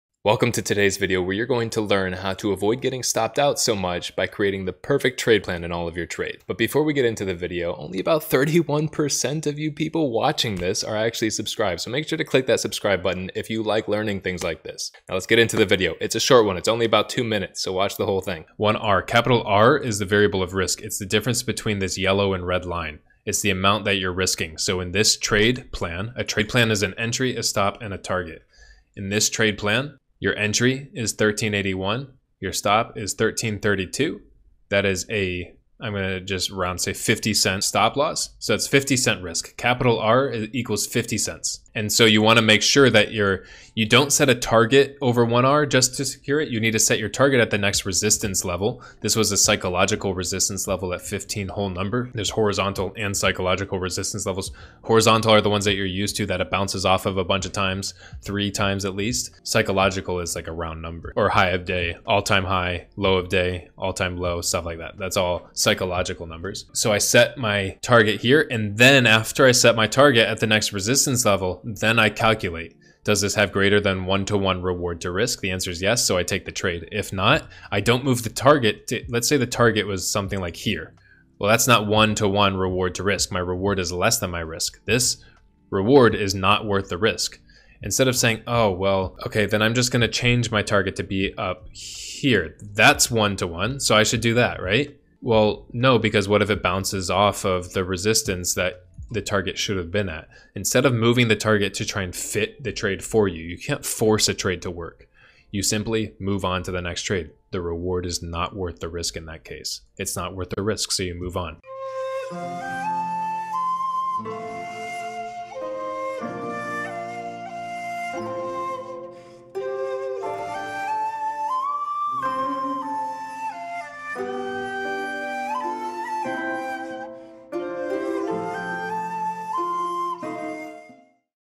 Getting stopped out on your trades? TRY THIS!
A quick and simple lesson for beginners on how to calculate your Reward-Risk Ratio and use it to effectively manage your trades - allowing you to minimize your losses and maximize your wins.

YouTube's algorithm has led to videos not being shown to all of a channel's subscribers.

0:00 - Intro
0:06 - Important message!
0:20 - Quick Lesson

Much love,
The Inlight Team